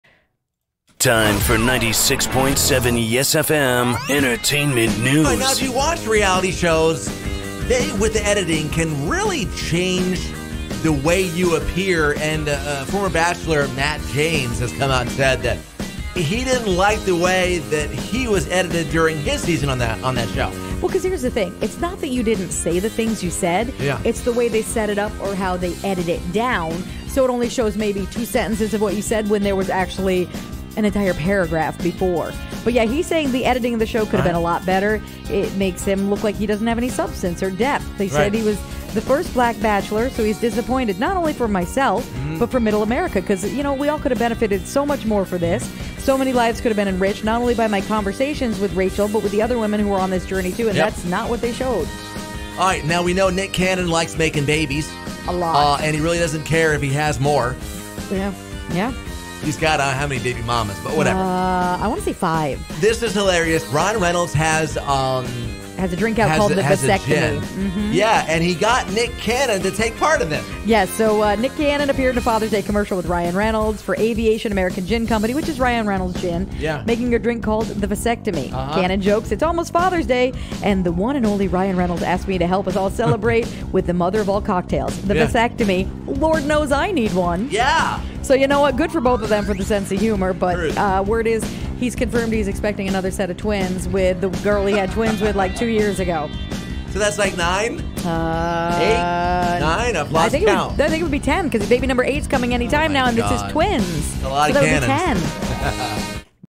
Bachelor Matt James not a fan of editing and Nick Cannon's gin appearance
Bachelor Matt James isn't thrilled with how he was edited in TheBachelor! And, NickCannon has a great cameo for RyanReynold's gin! A classic!